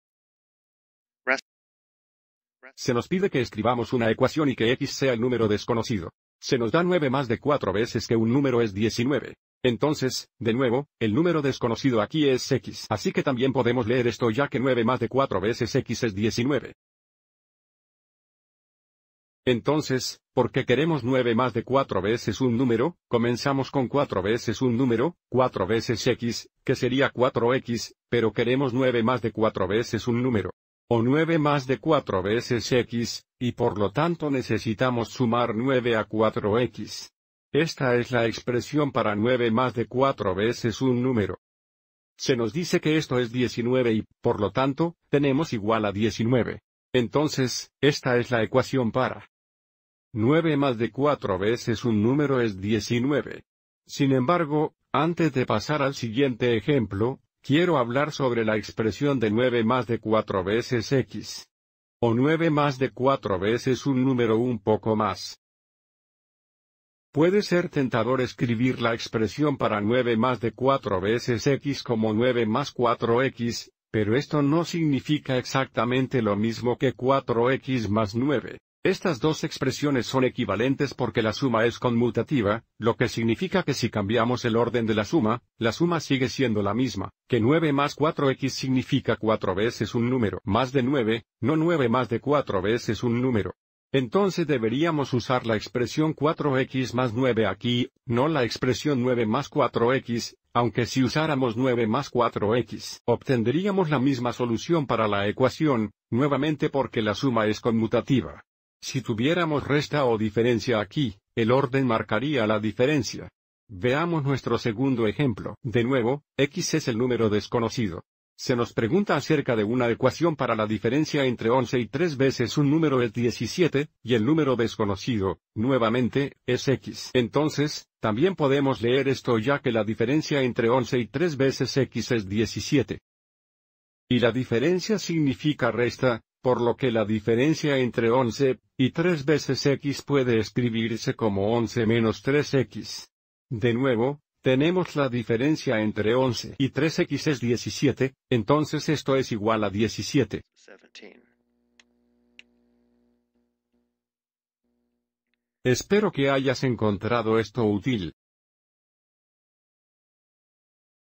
Ej 2: Escribe una ecuación lineal en una variable de una oración
Este video explica cómo escribir una ecuación lineal en una variable de una oración.